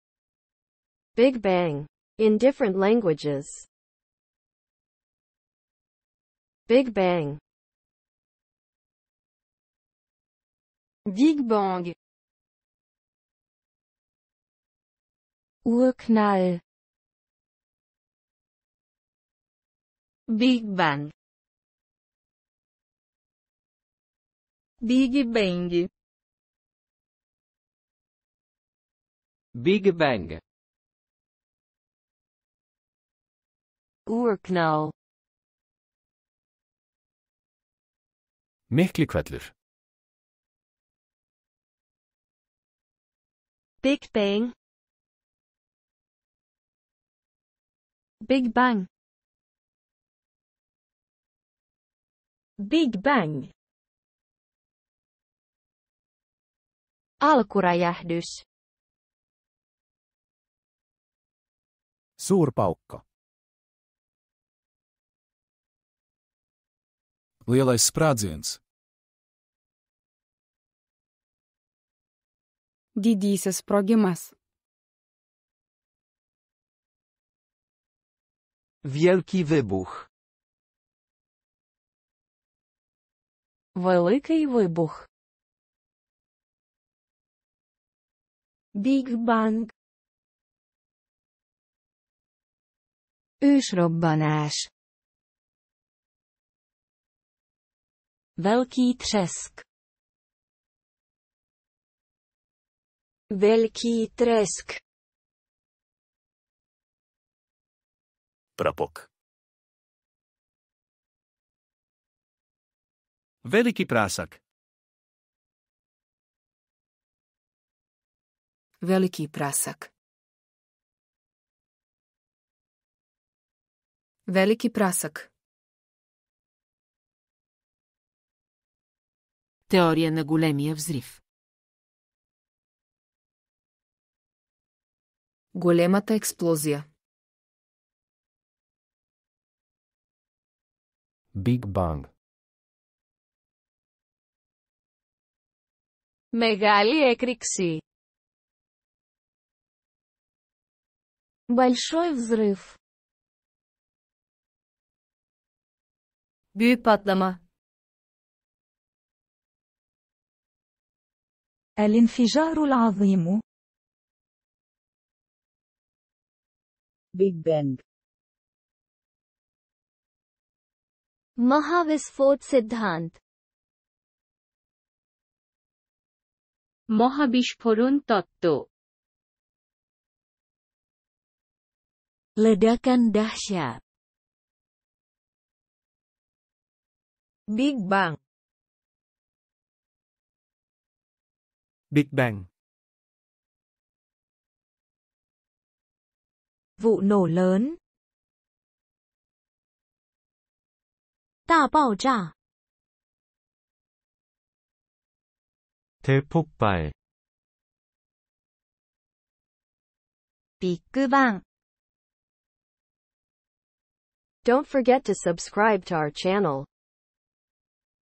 Big Bang (Astronomy) in Different Languages
Fone TV
Big Bang - English language (English language)
Big Bang - French language (Français)
Urknall - German language (Deutsche Sprache)
Big Bang - Spanish language (Idioma español)
Big Bang - Portuguese language (Língua portuguesa)
Big Bang - Italian language (Lingua italiana)
Oerknal - Dutch language (Nederlands)
Miklihvellur - Icelandic language (Íslenska)
Big Bang - Danish language (Dansk)
Big Bang - Norwegian language (Norsk)
Big bang - Swedish language (Svenska)
Alkuräjähdys - Finnish language (Suomen kieli)
Suur Pauk - Estonian language (Eesti keel)
Lielais sprādziens - Latvian language (Latviešu valoda)
Didysis sprogimas - Lithuanian language (Lietuvių kalba)
Wielki Wybuch - Polish language (Język polski)
Великий вибух - Ukrainian language (Українська мова)
Big Bang - Romanian language (Limba română)
Ősrobbanás - Hungarian language (Magyar nyelv)
Velký třesk - Czech language (Čeština)
Veľký tresk - Slovak language (Slovenčina)
Prapok - Slovene language (Slovenščina)
Veliki prasak - Croatian language (Hrvatski jezik)
Veliki prasak - Bosnian language (Bosanski jezik)
Велики прасак - Serbian language (Српски језик)
Теория на Големия взрив - Bulgarian language (Български език)
Големата експлозија - Macedonian language (Македонски јазик)
Big Bang - Albanian language (Gjuha shqipe)
Μεγάλη Έκρηξη - Greek language (Ελληνική γλώσσα)
Вялікі выбух - Belarusian language (Беларуская мова)
Большой взрыв - Russian language (Русский язык)
Büyük Patlama - Turkish language (Türkçe)
الانفجار العظيم - Arabic (اللغة العربية)
بگ بینگ - Urdu (اردو)
महाविस्फोट सिद्धान्त - Hindi (हिन्दी)
মহাবিস্ফোরণ তত্ত্ব - Bengali language (বাংলা ভাষা)
Ledakan Dahsyat - Indonesian language (Bahasa Indonesia)
Big Bang - Filipino language (Wikang Filipino)
บิกแบง - Thai language (ภาษาไทย)
Vụ Nổ Lớn - Vietnamese language (Tiếng Việt)
大爆炸 - Chinese language (汉语)
대폭발 - Korean language (한국어)
ビッグバン - Japanese language (日本語)